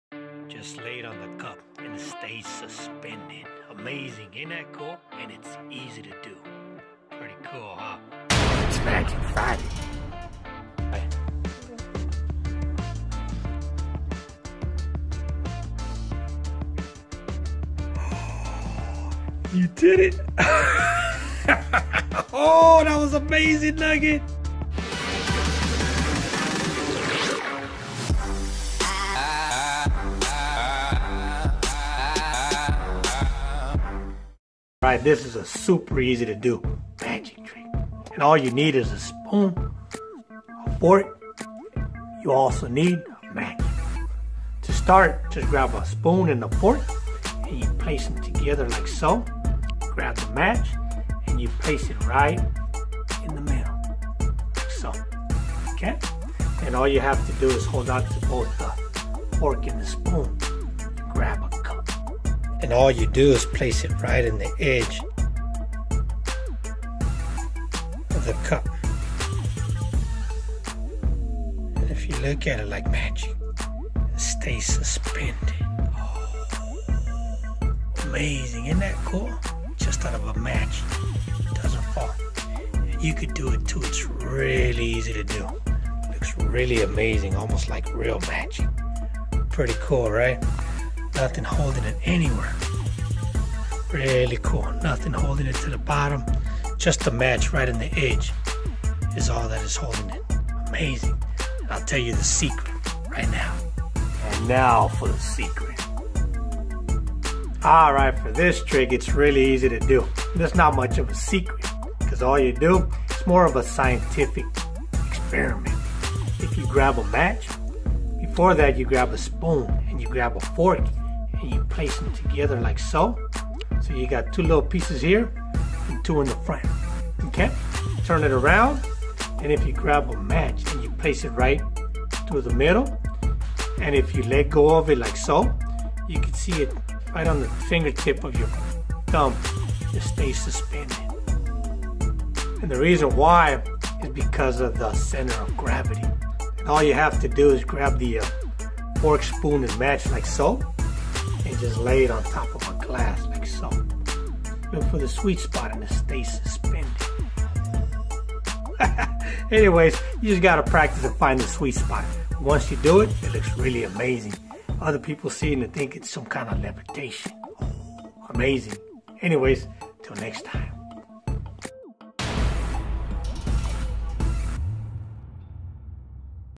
IMPOSSIBLE LEVITATION.. Easy to do.!
Balance a spoon and fork on a match or toothpick on a cup! it appears to defy gravity.. Is it Gravity Levitation or Suspension? It suspends in mid air Balanced on the tip of the cup or finger...